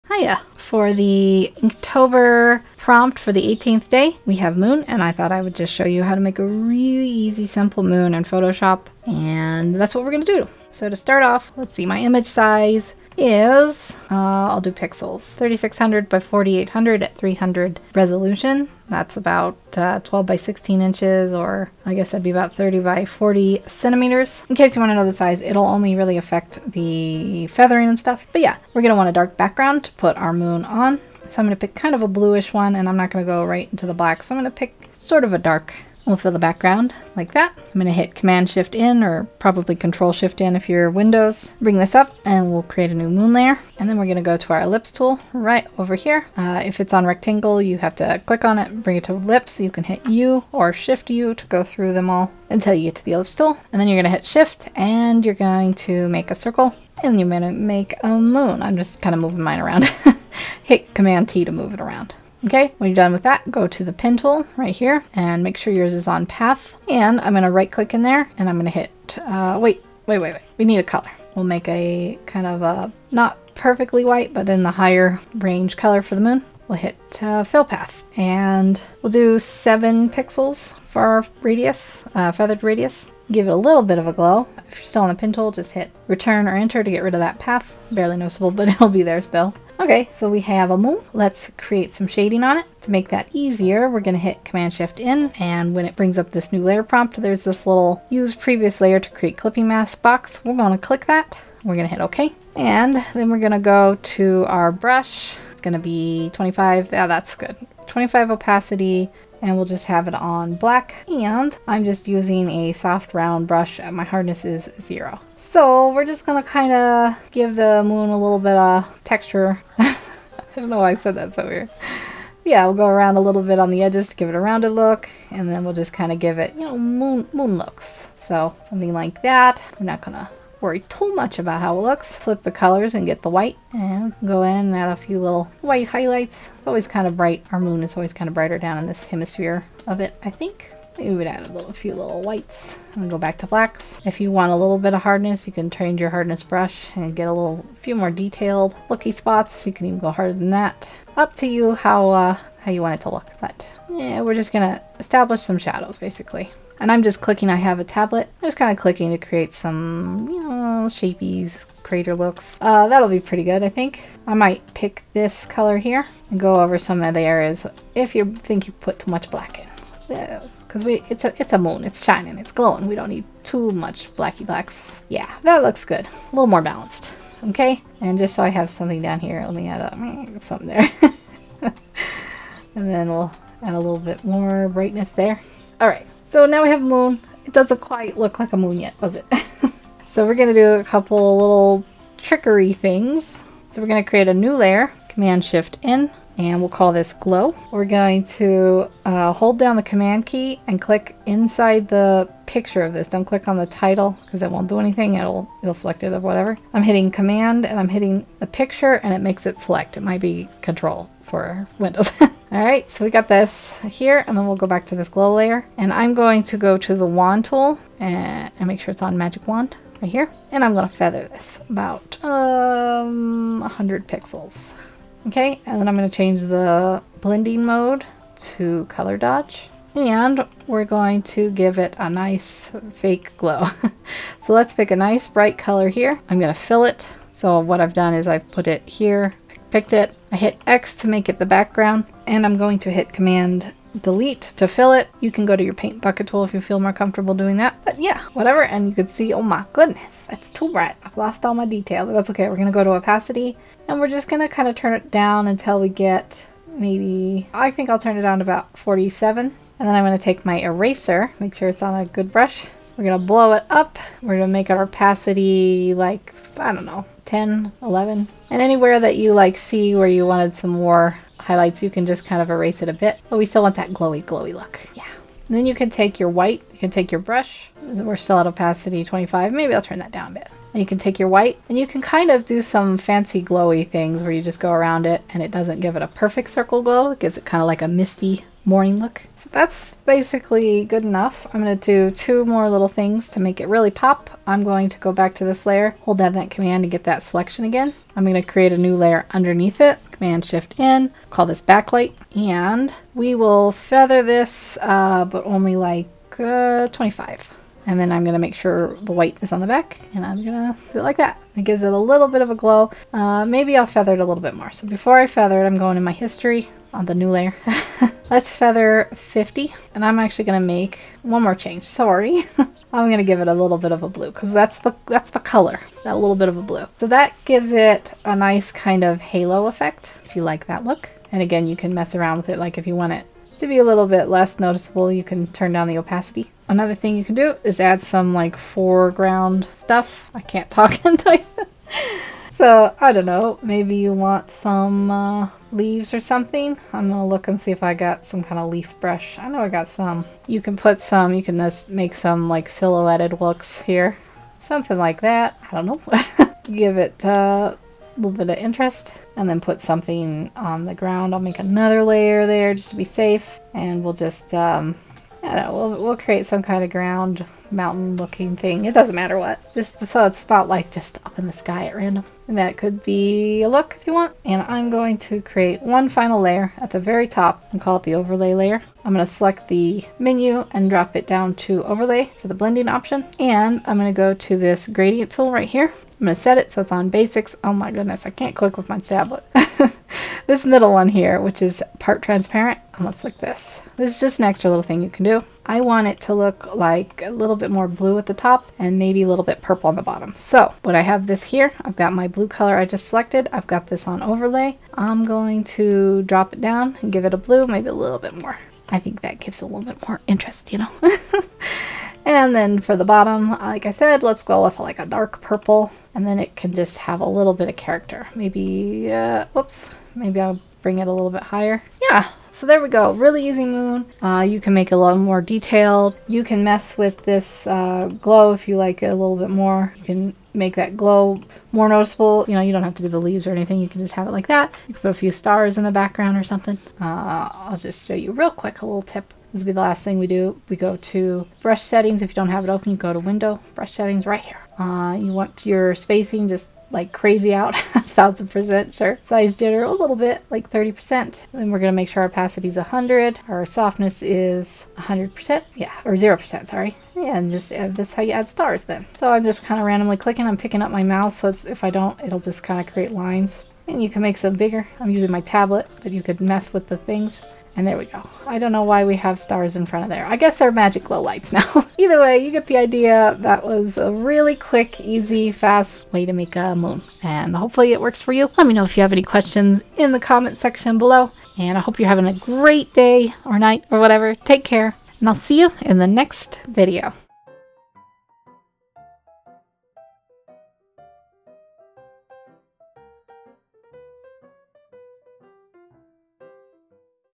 Moon - Inktober day 18 | How to Make a Simple Moon In Photohsop
Quickie tutorial on how to make an easy moon in photoshop.